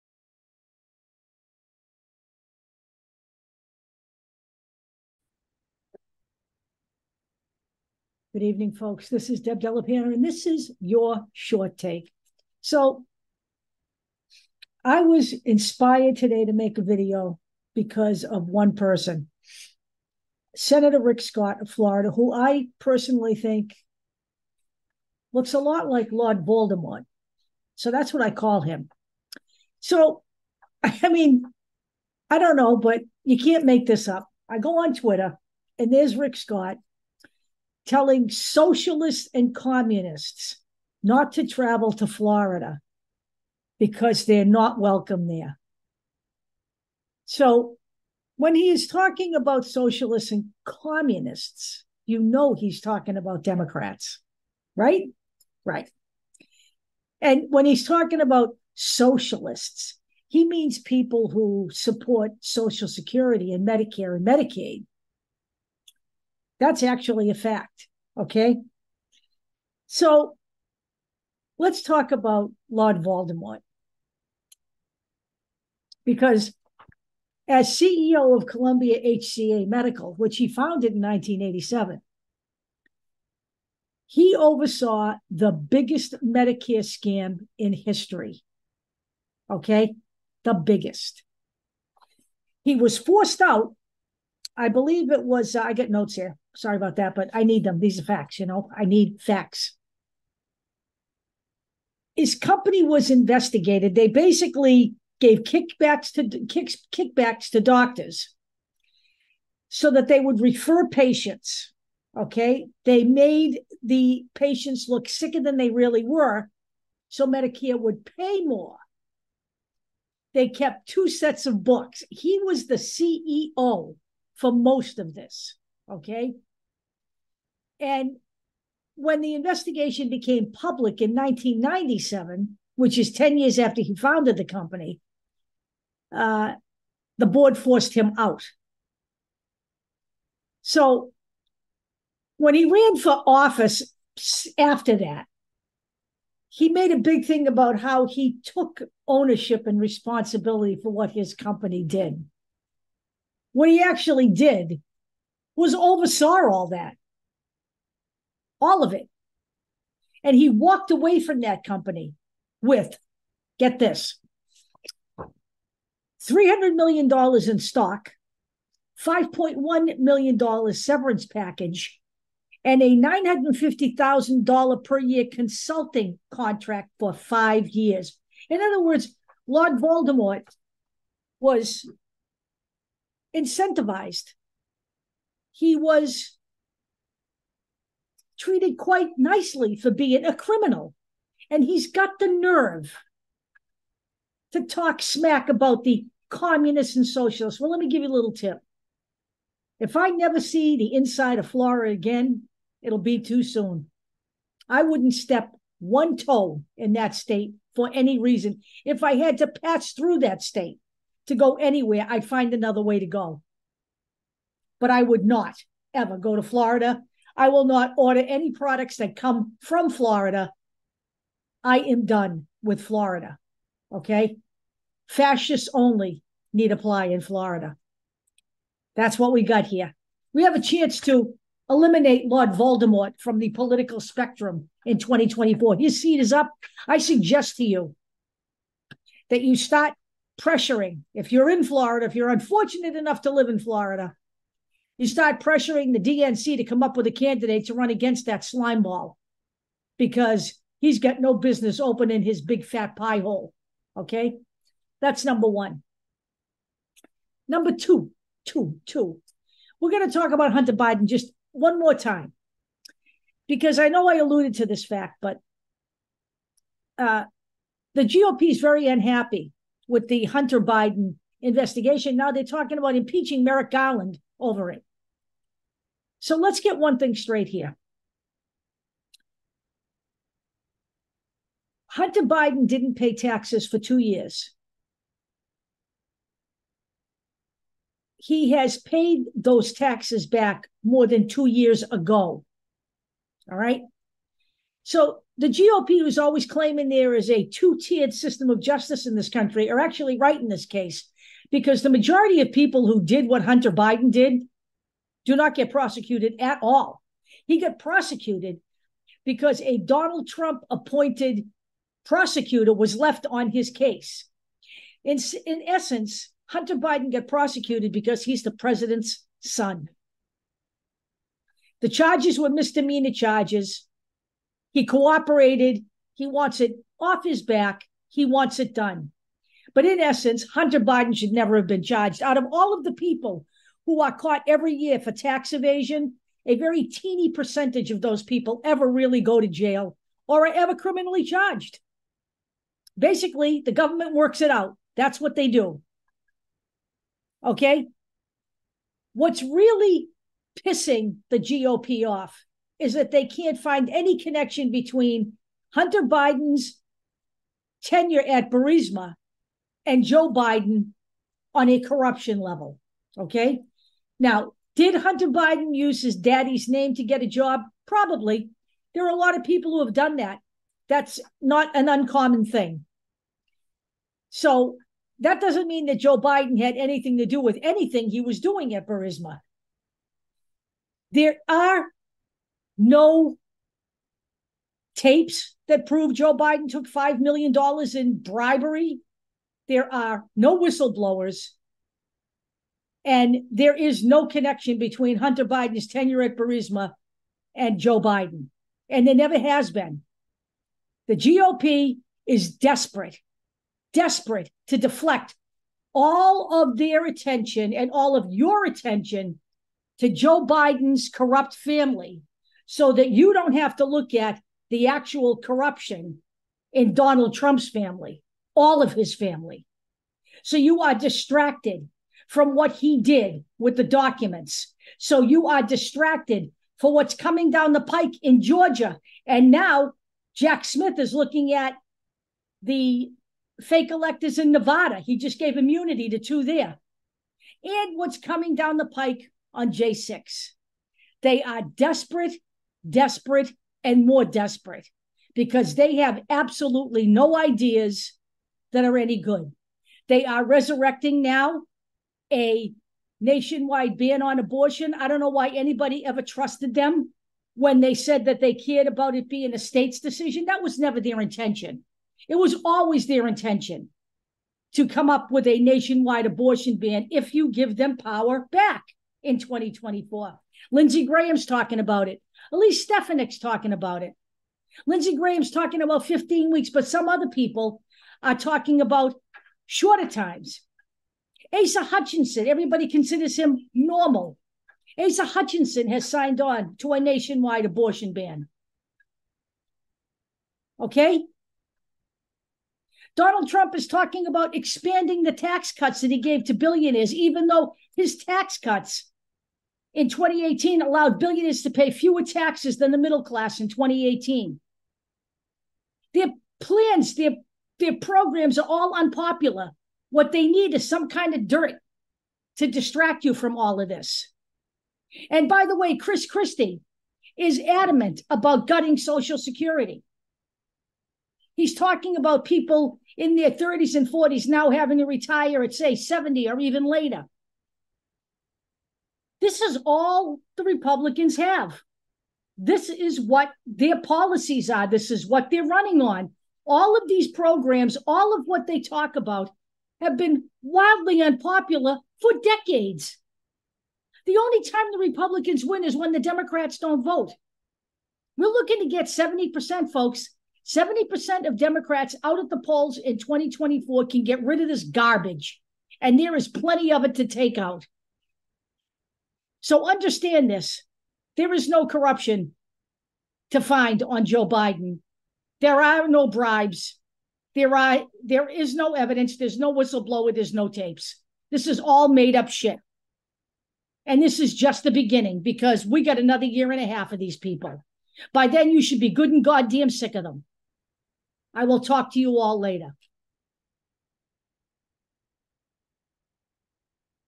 Short Takes-Ruminations on Lord Voldemort, Hunter & Joe Biden, the Pathetic GOP, June 27, 2023
Good evening, folks. You know, in years past, the GOP could be made fun of and brushed off. But don't kid yourselves, those days are gone no matter how ridiculous the MAGA cult looks. Every day they are in office, they are a danger to democracy.

Tonight, we talk about Rick Scott, also known as Lord Voldemort, and his pathetic Tweet about "socialists and communists" staying out of his state...what he calls The Sunshine State. Don't worry, Rick. None of us want to go within three hundred miles of that fascist shithole.

But he really has a whole lot of chutzpah to talk about "socialist" policies like Social Security and Medicare, since he was the criminal CEO whose firm was at the heart of the biggest Medicare fraud in history.

We also talk about Hunter and Joe Biden, and the GOP's desperate attempt to find any kind of corruption at all between them.

And finally, the pathetic platform of the GOP and what they have in store for YOUR future should you allow them back in power. A word to the wise: DON'T. In case you missed it in all the excitement over Trump's indicment:

That's it for tonight, gang. But we'll be back because there's a lot more on our white board.